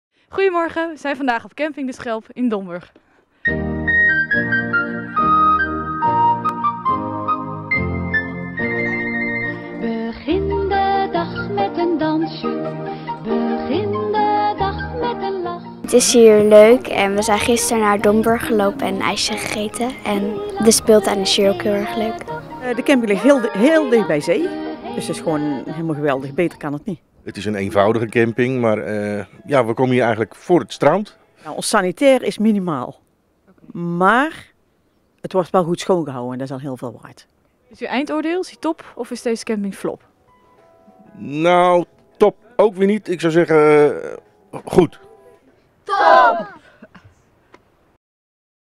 Goedemorgen vanaf camping De Schelp in Domburg!
Vandaag gaan we op visite bij minicamping De Schelp in Domburg. Wat vinden de campinggasten van de camping?